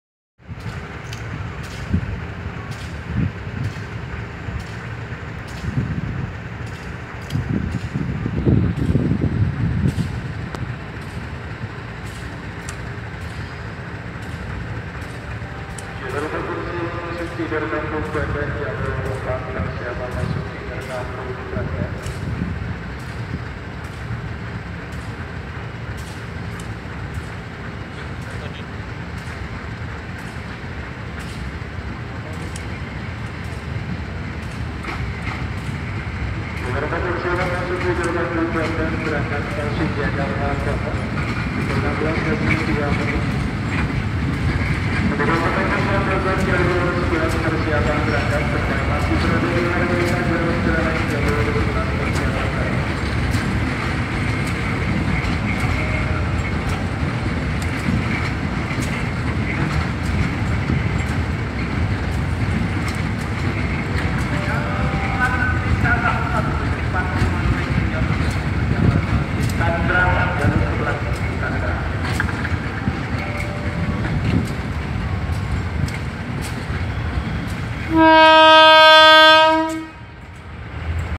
KRL KFW i9000 Train Set 03 Tiba Stasiun Jakarta Kota
● Video direkam pada tanggal 15 Oktober 2016.

Selamat menonton...
Terima kasih.